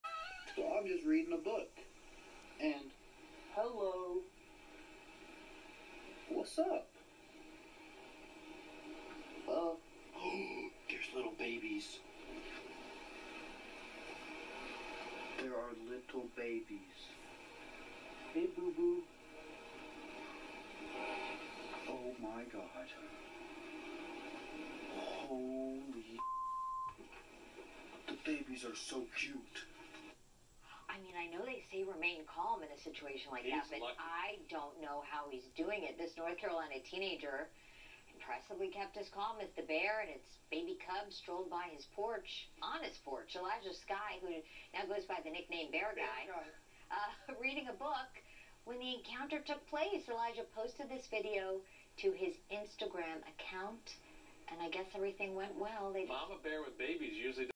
Bear and baby cubs decide to hang out
A young man reading a book on his porch discovered a bear and her cubs behind him.  KTLA 5 reports